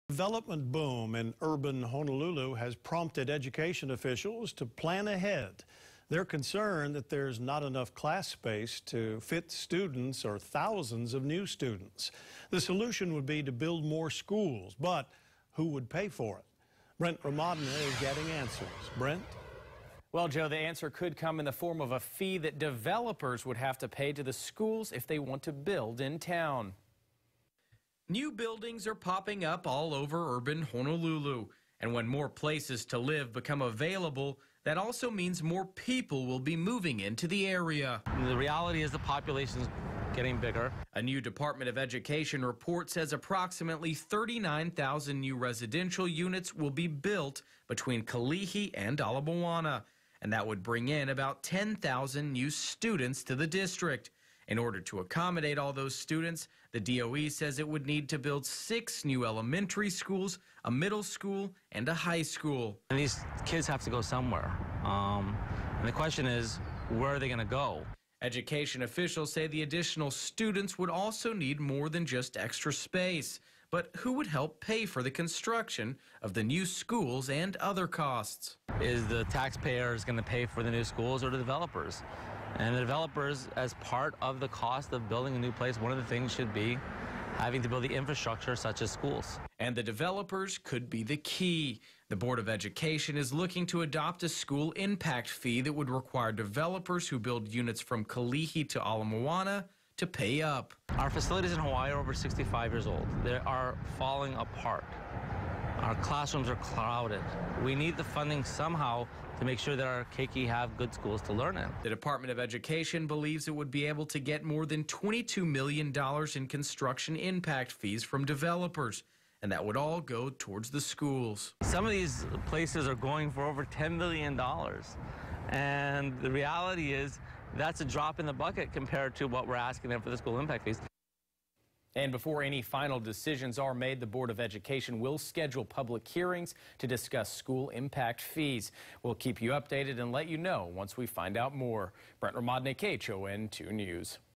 More development means more residents, but can the schools handle the increase?
Now with the rail and more developments on the way the Board of Education is taking a look at how to handle the thousands of expected students that will soon move into the city.